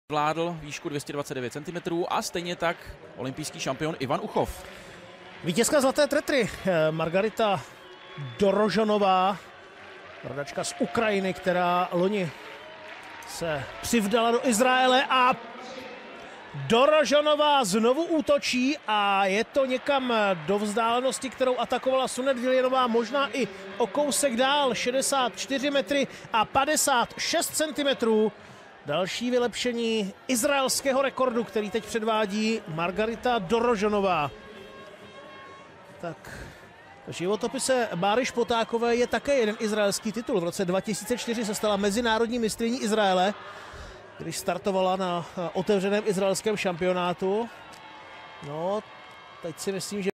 Margarita DOROZHON. Israeli National Record in javelin throw 64 meters 56!
Israeli national record in javelin throw 64 meters 56! Diamond League 2015 Oslo.